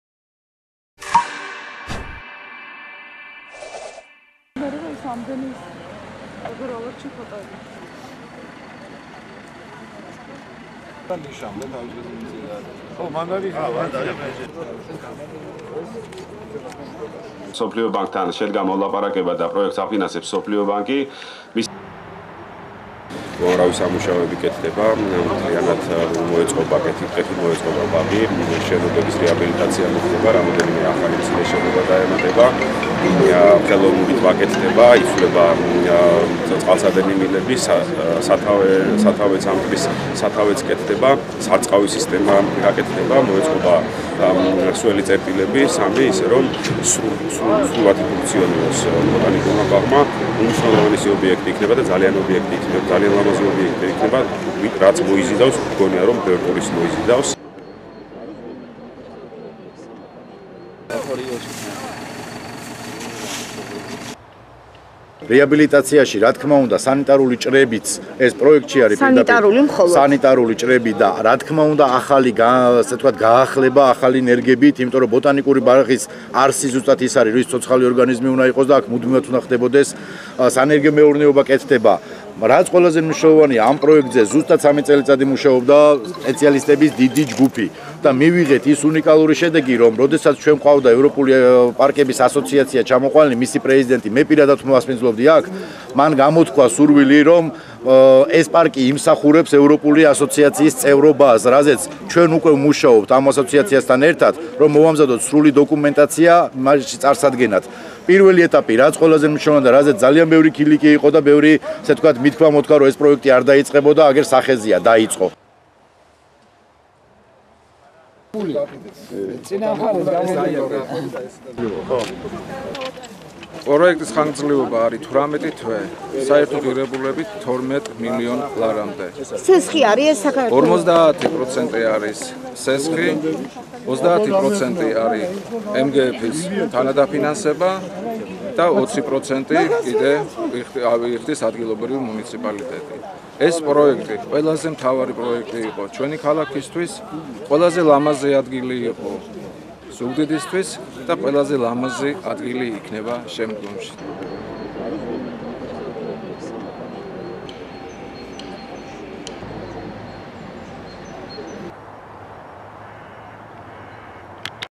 ბაღის რეკონსტრუქცია დაიწყო
მეცხრამეტე საუკუნეში გაშენებული და მოუვლელობის გამო განადგურებული ზუგდიდის ბოტანიკური ბაღის რეკონსტრუქცია მრავალწლიანი დაპირებების შემდეგ დაიწყო.